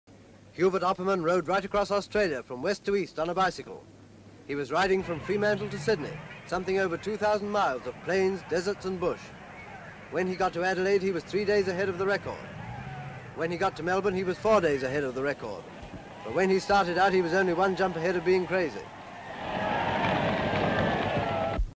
Cyclist breaks record riding across Australia (1938)
Hubert Opperman (Australian cyclist and politician) breaks record riding from Freemantle to Sydney

Full Description:

AUSTRALIA: Various:
EXT

BICYCLES Hubert Opperman breaks Fremantle-Sydney Record.
HUBERT OPPERMAN BREAKS FREEMANTLE TO SYDNEY RECORD ON A BICYCLE:

OPPERMAN, Hubert Breaks Fremantle - Sydney Cycle record.

SKI-ING Ski-Jumping at Cary Illinois, H. Sorensen, Sigmund & Birger Ruud.

Cycling; Bicycles; Personalities - Sport

recordbreaker, record breaking, bicycling, biking
Background: Hubert Opperman (Australian cyclist and politician) breaks record riding from Freemantle to Sydney